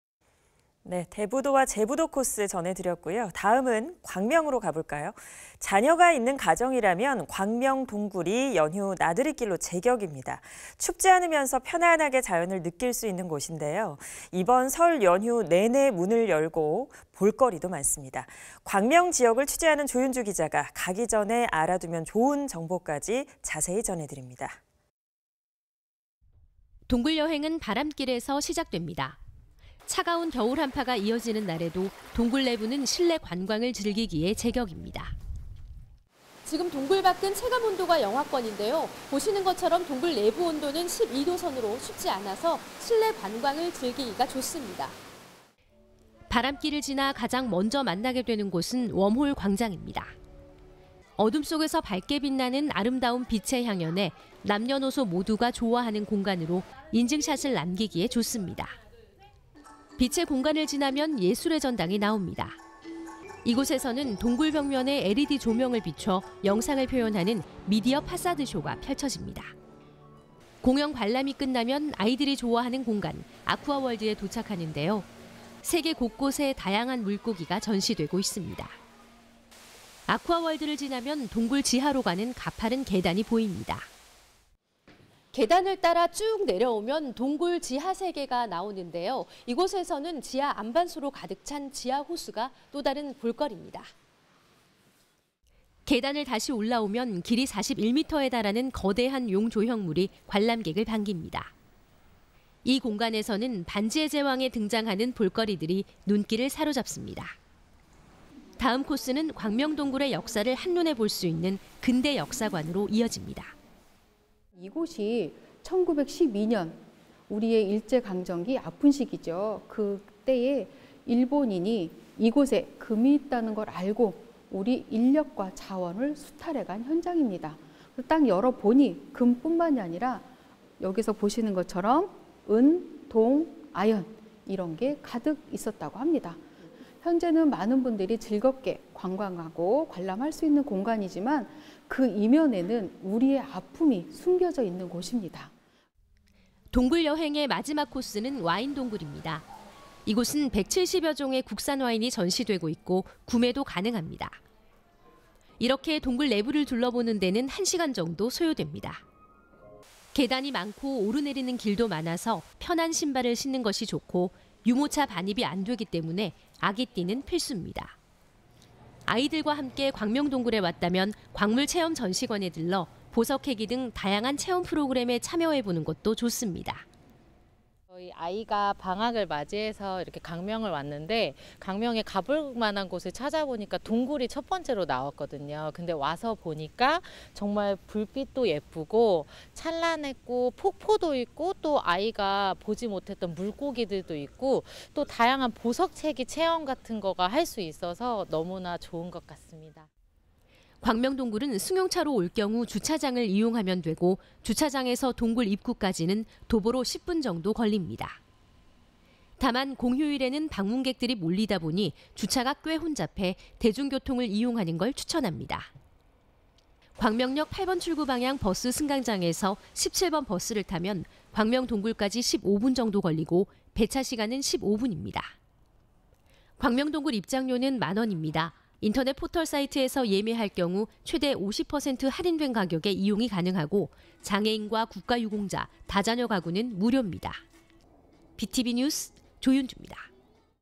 (설 특집) "겨울에도 춥지 않아요" 가족과 즐기는 '광명동굴'
[앵커멘트]

자녀가 있는 가정이라면 광명동굴이
연휴 나들잇길로 제격입니다.

춥지 않으면서 편안하게 자연을 느낄 수 있는 곳인데요.

이번 설 연휴 내내 문을 열고, 생각보다 볼거리도 많습니다.

광명 지역을 취재하는 조윤주 기자가 가기 전에 알아두면
좋은 정보까지 자세히 전해드립니다.

[리포트]

동굴 여행은 바람길에서 시작됩니다.

차가운 겨울 한파가 이어지는 날에도 동굴 내부는
실내 관광을 즐기기에 제격입니다.

"지금 동굴 밖은 체감온도가 영하권인데요.
보시는 것처럼 동굴 내부 온도는 12도 선으로
춥지 않아서 실내 관광을 즐기기 좋습니다."

바람길을 지나 가장 먼저 만나게 되는 곳은
'웜홀 광장' 입니다.

어둠 속에서 밝게 빛나는 아름다운 빛의 향연에
남녀노소 모두가
좋아하는 공간으로 인증샷을 남기기에 좋습니다.

빛의 공간을 지나면 예술의 전당이 나옵니다.
이 곳에서는 동굴벽면에 LED조명을 비춰 영상을 표현하는
미디어 파사드쇼가 펼쳐집니다.

공연 관람이 끝나면 아이들이 좋아하는 공간,
아쿠아월드에 도착하는데요.
세계 곳곳의 다양한 물고기가 전시되고 있습니다.

아쿠아월드를 지나면 동굴 지하로 가는 가파른 계단이 보입니다.

"계단을 따라 쭉 내려오면 동굴 지하세계가 나오는데요.

이곳에서는 지하 암반수로 가득 찬
지하 호수가 또 다른 볼거립니다."

계단을 다시 올라오면,
길이 41m에 달하는 거대한 용 조형물이
관람객을 반깁니다.

이 공간에서는 반지의 제왕에 등장하는 볼거리들이
눈길을 사로잡습니다.

다음 코스는 광명동굴의 역사를 한눈에 볼 수 있는
근대역사관으로 이어집니다.

[ 정옥희 / 광명시 문화관광해설사 ]
"이곳은 1912년 우리의 일제강점기 아픈 시기죠.
일본이 이곳에 금이 있다는 걸 알고 우리의 인력과 자원을
수탈해 간 현장입니다. 열어보니 금뿐만 아니라 보시는 것처럼
은, 동, 아연이 가득 있었다고 합니다.
현재는 많은 분들이 즐겁게 관광하고 관람할 수 있는 공간이지만,
그 이면에는 우리의 아픔이 숨겨져 있는 곳입니다."

동굴여행의 마지막 코스는 와인동굴입니다.

이 곳은 170여 종의 국산 와인이 전시되고 있고,
구매도 가능합니다.

이렇게 동굴 내부를 둘러보는데는 1시간 정도 소요됩니다.

계단이 많고 오르내리는 길도 많아서
편한 신발을 신는 것이 좋고
유모차 반입이 안되기 때문에
아기띠는 필수입니다.

아이들과 함께 광명동굴에 왔다면
광물체험전시관에 들러
보석캐기 등 다양한 체험프로그램에
참여해 보는 것도 좋습니다.

[ 장은영 / 전라북도 전주시 ]
"아이가 방학을 맞이해서 광명을 왔는데
광명에 가볼 만한 곳을 찾아보니까 동굴이 첫 번째로 나왔거든요.
와서 보니까 정말 불빛도 예쁘고 찬란했고 폭포도 있고
아이가 보지 못했던 물고기들도 있고
다양한 보석 캐기 체험도 할 수 있어서 너무나 좋은 것 같습니다."

광명동굴은 승용차로 올 경우 주차장을 이용하면 되고
주차장에서 동굴 입구까지는 도보로 10분 정도 걸립니다.

다만, 공휴일에는 방문객들이 몰리다보니 주차가 꽤 혼잡해
대중교통을 이용하는 걸 추천합니다.

광명역 8번출구 방향 버스 승강장에서 17번 버스를 타면
광명동굴까지 15분 정도 걸리고, 배차 시간은 15분 입니다.

광명동굴 입장료는 1만원입니다.
인터넷 포털사이트에서 예매할 경우 최대50% 할인된 가격에

이용이 가능하고, 장애인과 국가유공자,
다자녀 가구는 무료 입니다.

B tv 뉴스 조윤주입니다.

[영상취재 이두원]